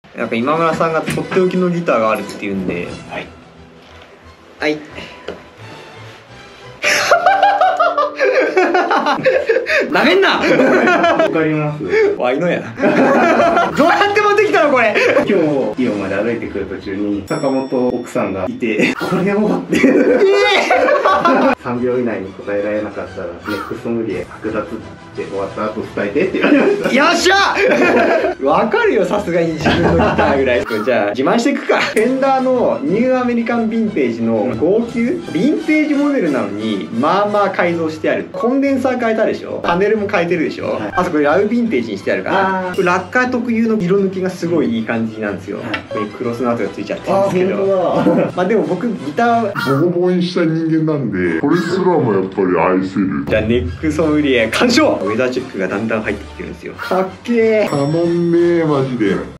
目隠しで自分のギターを当てさせられる楽器店員 Part.56
ご案内コード　「22861」

■島村楽器甲府昭和店　ホームページURL

スタッフ：坂本、今村